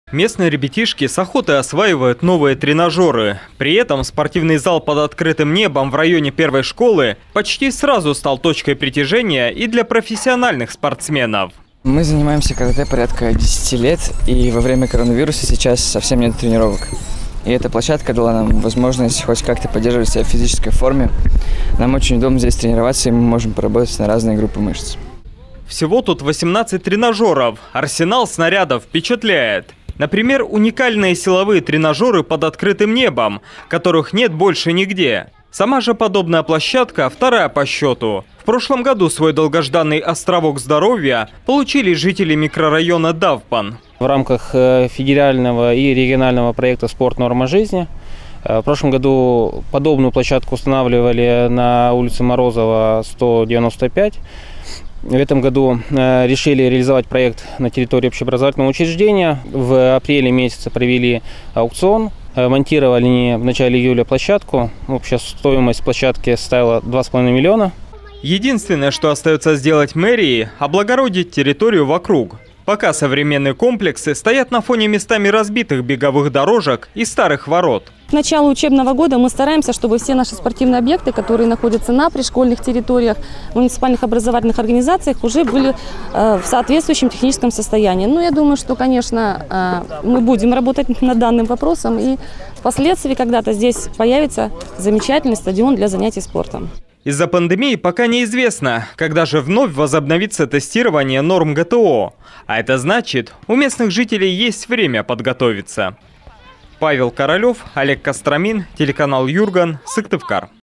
В Сыктывкаре появилась уникальная спортплощадка
В центре Сыктывкара заработала новая специализированная площадка с возможностью проводить на ней тестирование на нормы ГТО. 18 тренажёров под открытым небом круглосуточно ждут всех желающих. Бесплатно и доступно.